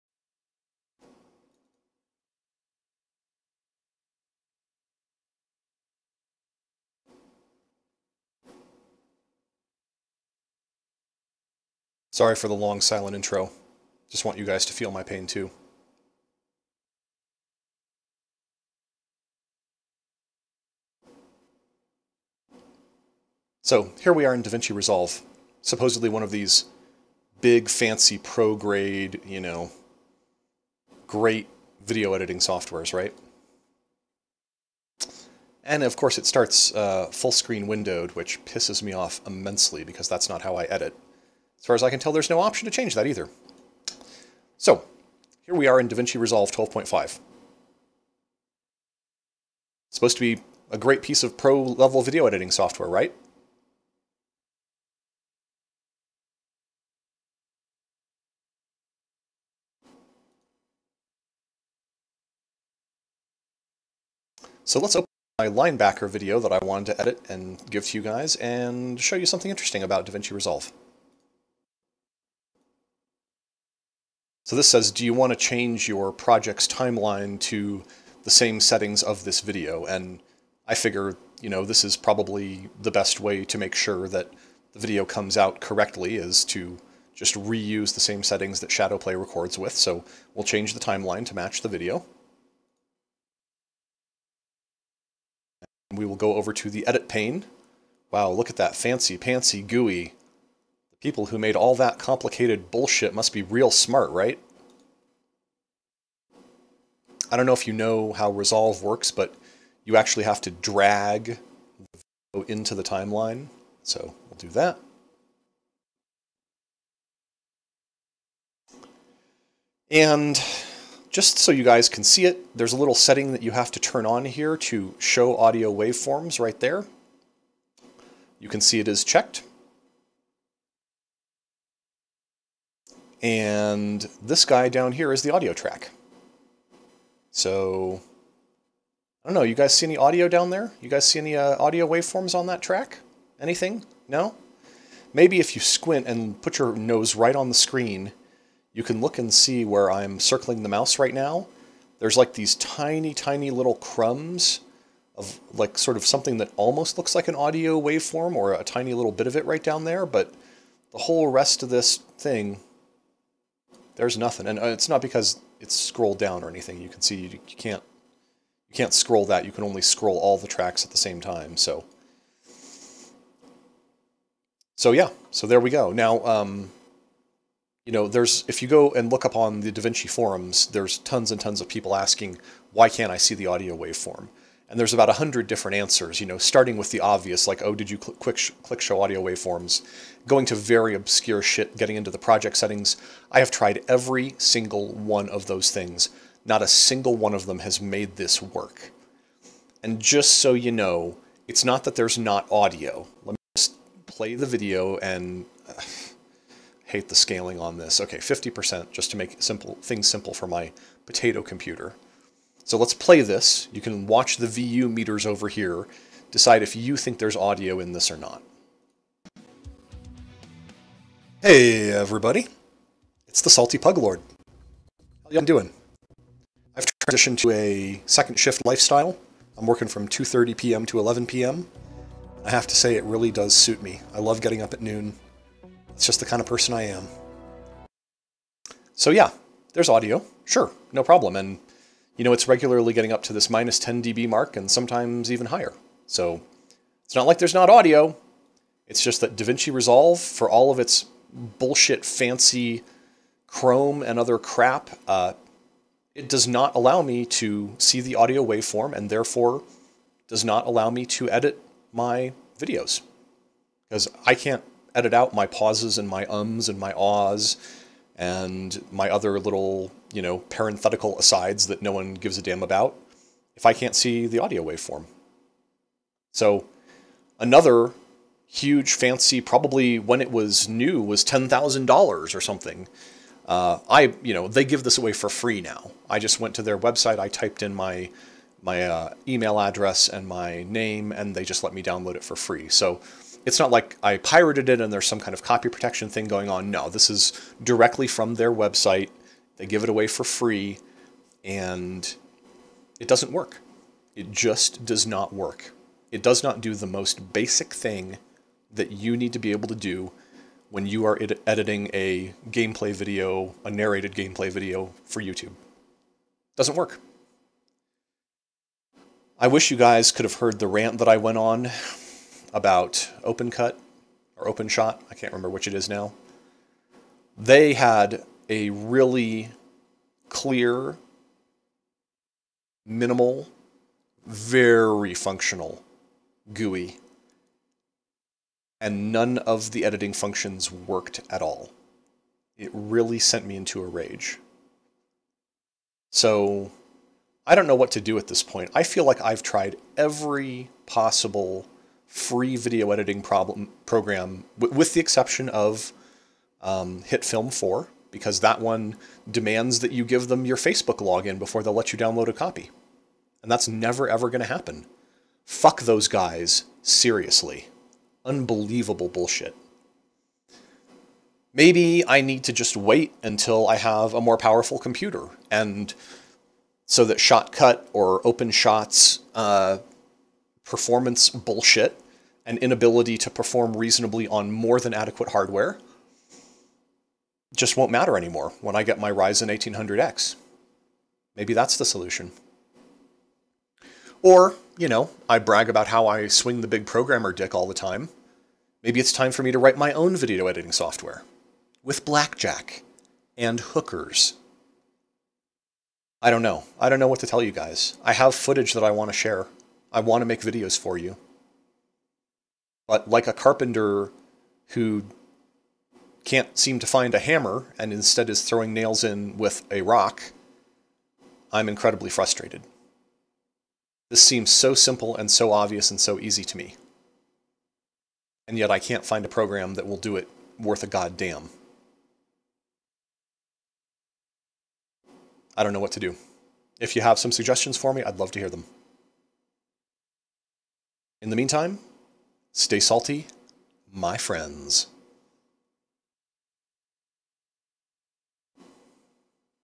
How DaVinci Resolve 12.5 is useless for YTers.
This is really depressing, y'all. PLEASE - if you have a suggestion for *free* video editing software *that actually works* let me know. I've tried: Shotcut, VSDC Free,  OpenShot, DaVinci 12.5.

The following features are non-negotiable:

- Show audio waveforms while editing footage recorded with Shadowplay.

- Respond to UI inputs in less than 50ms on an Athlon X2 6400+ with 6GB of RAM. (No "we wrote our UI in Python" horseshit like OpenShot.)

Hitfilm will not be considered. Fuck those guys in the ass - WITH A RAKE. Seriously.